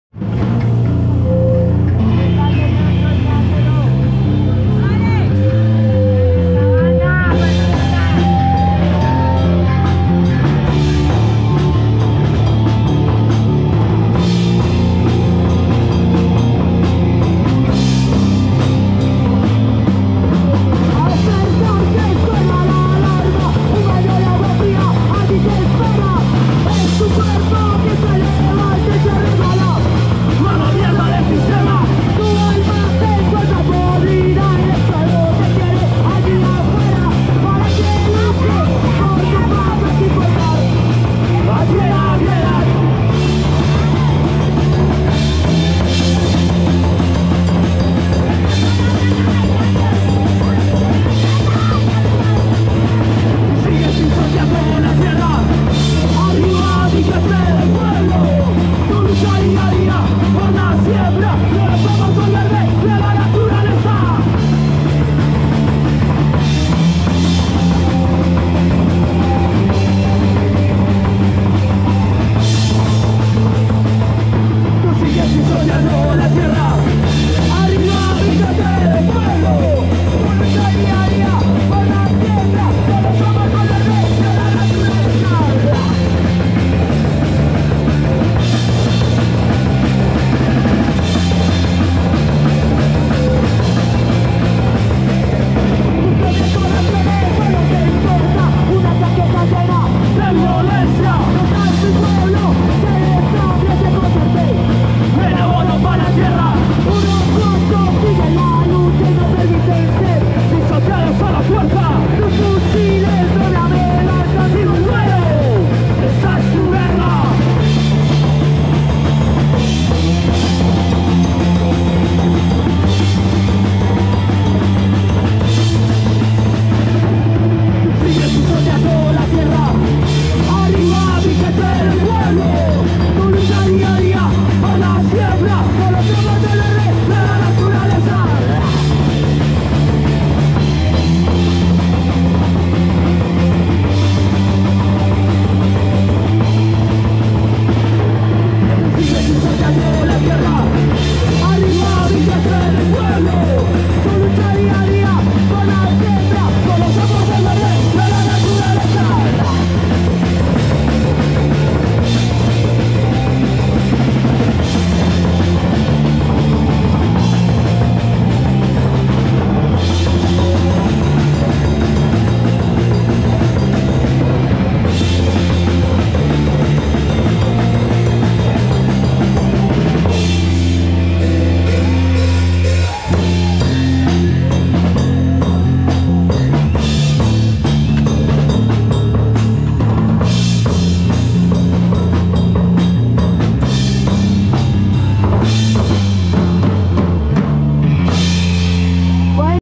Inkorrectos - La Chusma Baila Pogo 2(2024)(Punk - Video en Vivo)
Banda: Inkorrectos
Genero: Punk
Pais: Colombia (Bogota)
Evento: La Chusma Baila Pogo (2024)
Parque Nacional (Bogota - 24/02/24)